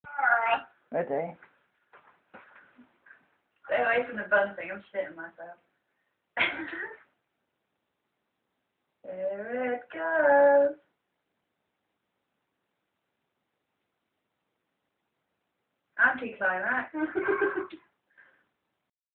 ''Indoor'' sparklers
video cuts out before the fire alarm and hiding.